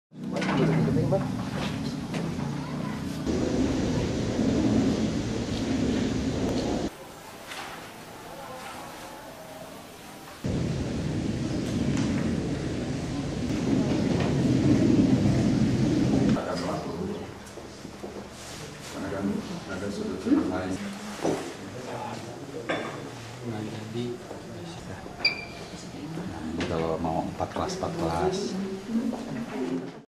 Antisipasi Kecurangan, 48 Kamera CCTV Pantau UN
Metrotvnews.com, Cimahi: Guna menghindari terjadinya kecurangan dalam pelaksanaan UN, panitia Ujian Nasional SMA Negeri 3 Kota Cimahi, Jawa Barat, Senin (14/4/2014), menggunakan 48 kamera CCTV untuk mengawasi para siswa yang sedang melaksanakan ujian.